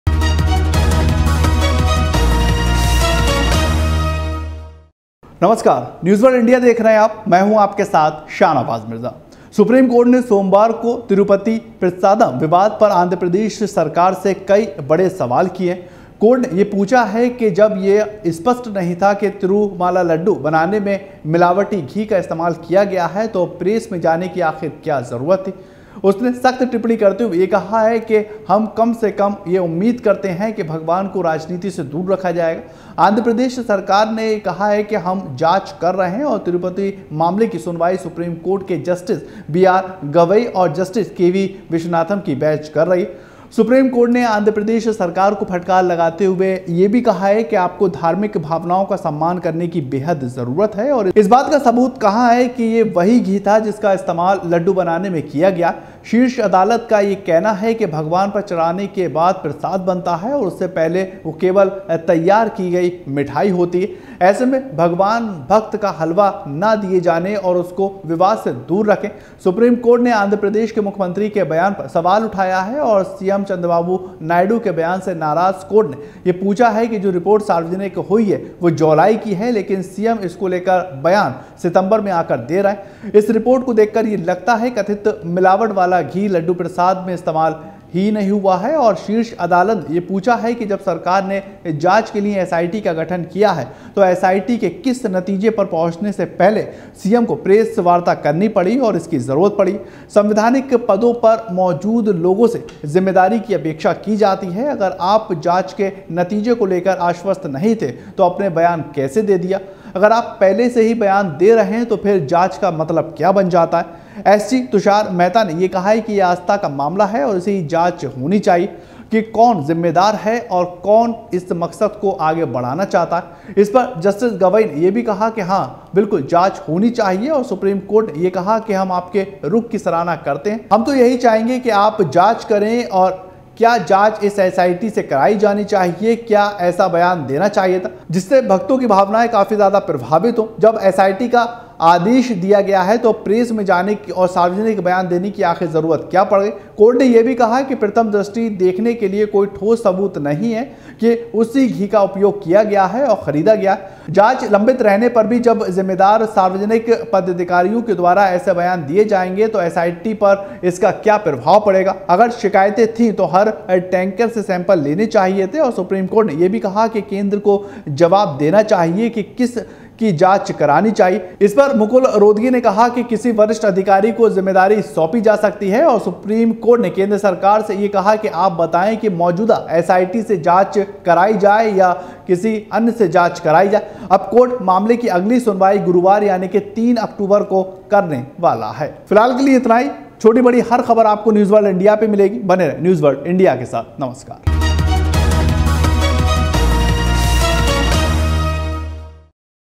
Tirupati Laddu Controversy: तिरुपति लड्डू विवाद में सीएम नायडू की टिप्पणियों पर उठे सवाल ?
सुप्रीम कोर्ट ने सोमवार को तिरुपति प्रसादम विवाद पर आंध्र प्रदेश सरकार से कई कड़े सवाल किए. कोर्ट ने पूछा कि जब यह स्पष्ट नहीं था कि तिरुमाला लड्डू बनाने में मिलावटी घी का इस्तेमाल किया गया था, तो प्रेस में जाने की क्या जरूरत थी? उसने सख्त टिप्पणी करते हुए कहा कहा कि कम से कम हम यह उम्मीद करते हैं कि भगवान को राजनीति से दूर रखें. आंध्र प्रदेश सरकार ने कहा कि हम जांच कर रहे हैं.